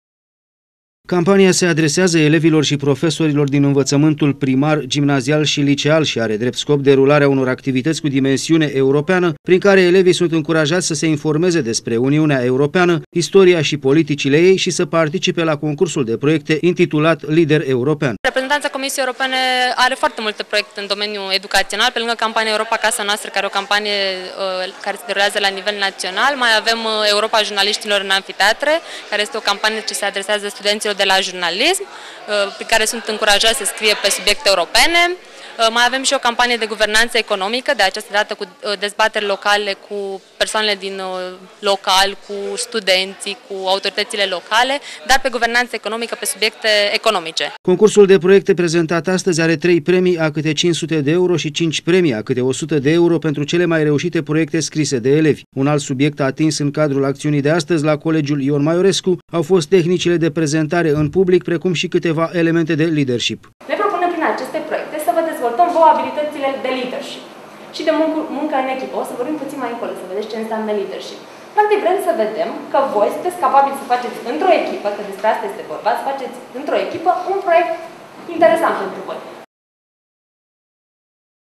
Concurs De Proiecte Europene
DEZVOLTAREA CALITĂŢILOR DE LEADER ESTE UNUL DINTRE OBIECTIVELE PE CARE REPREZENTANŢII COMISIEI EUROPENE ÎL DORESC ÎN RÂNDUL ELEVILOR. ÎN ACEST SENS, REPREZENTANŢA COMISIEI EUROPENE A LANSAT CEA DE-A DOUA EDITIE A CAMPANIEI DE INFORMARE SI COMUNICARE IN SCOLI SI LICEE "EUROPA, CASA NOASTRA". ELEVII COLEGIULUI NATIONAL ION MAIORESCU, AU FOST ASTAZI ÎNCURAJATI SA-SI DEZVOLTE CALITATILE ANTREPRENORIALE, PENTRU REALIZAREA UNOR PROIECTE.